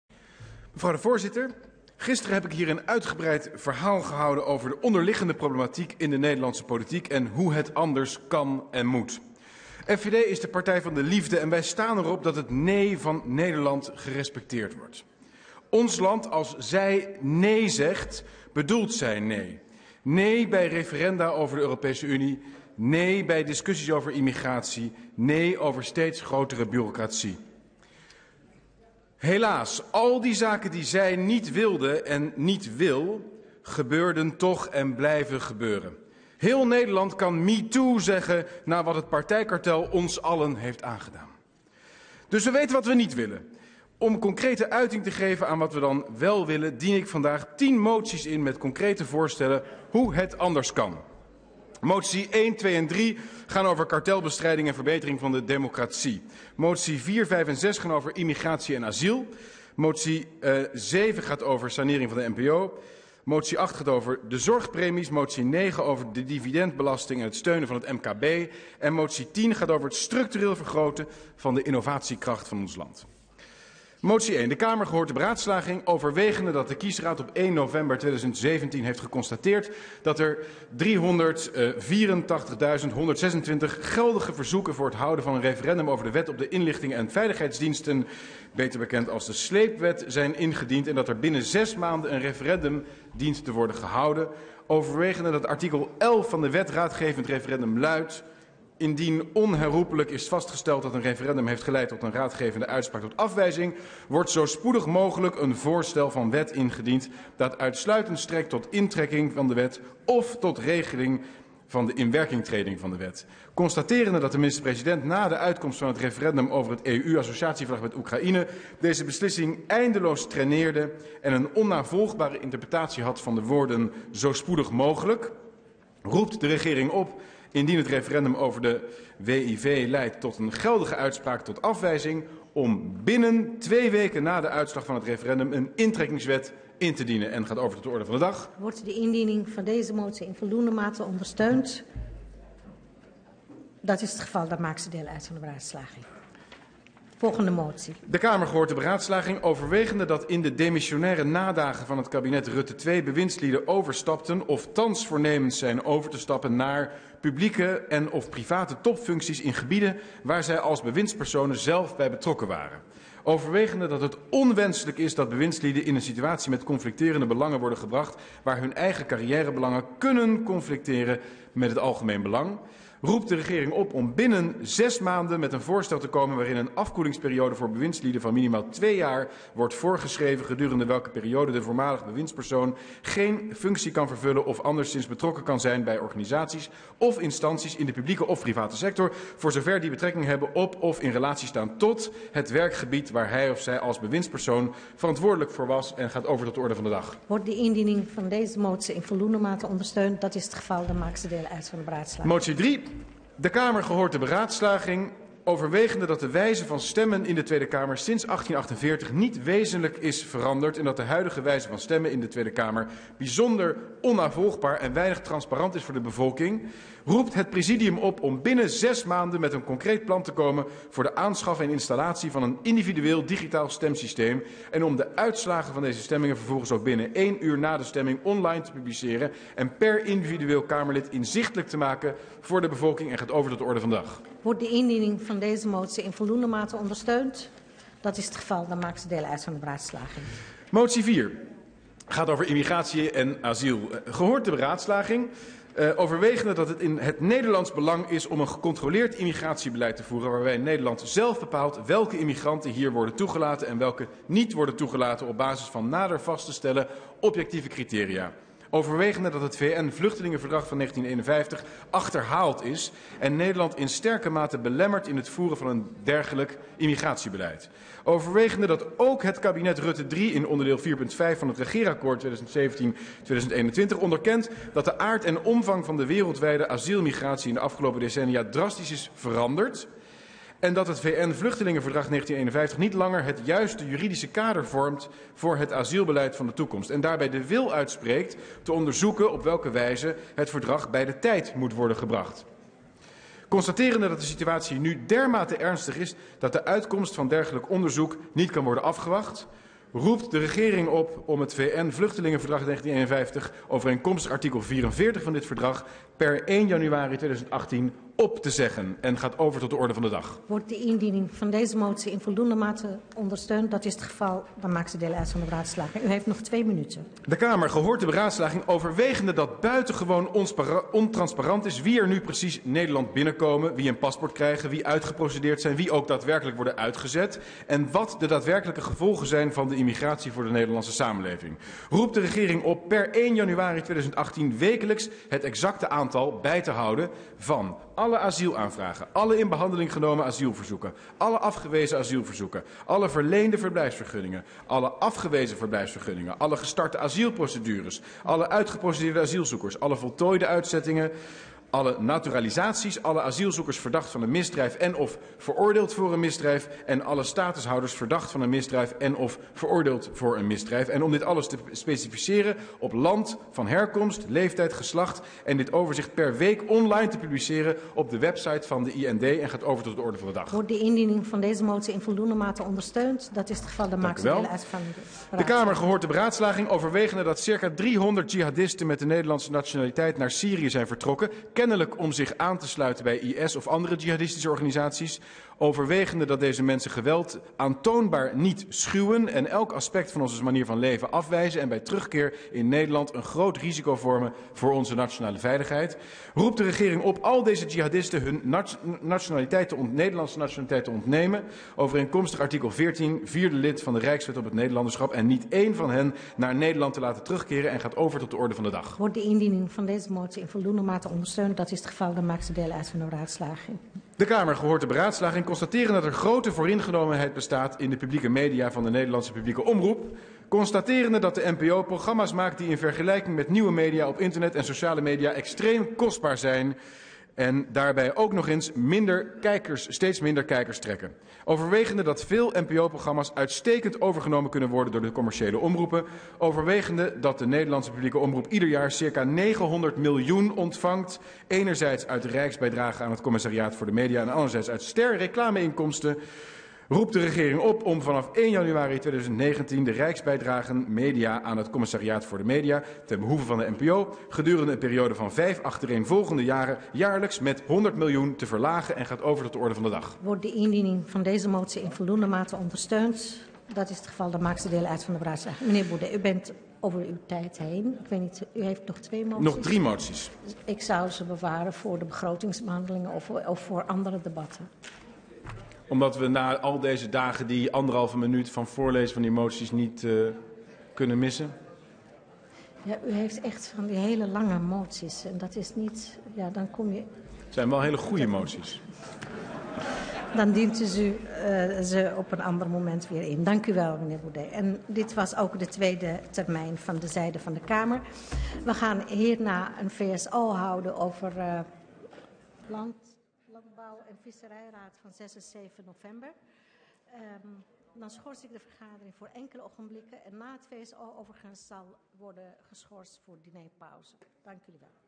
De 10 moties van Baudet
Na twee dagen non-stop debat over de regeringsverklaring wilde Thierry Baudet zojuist 10 FVD-moties indienen met concrete en constructieve voorstellen. Na motie nummer 7 maakte de voorzitter er abrupt een eind aan (2 november 2017).

✓ Word lid van FVD voor maar €25 per jaar